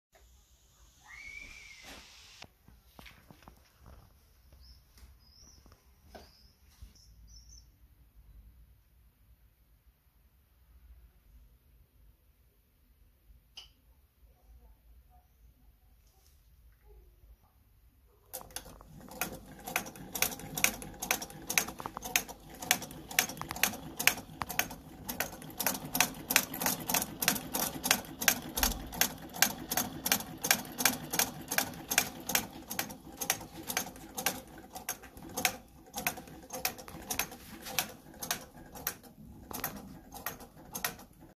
PFAFF 11 pierwsze szycie Vintage Old sewing machine
Obiecałem więc jest😀. Pierwszy test tej maszyny. Mile zostałem zaskoczony. Szyje bardzo ładnie.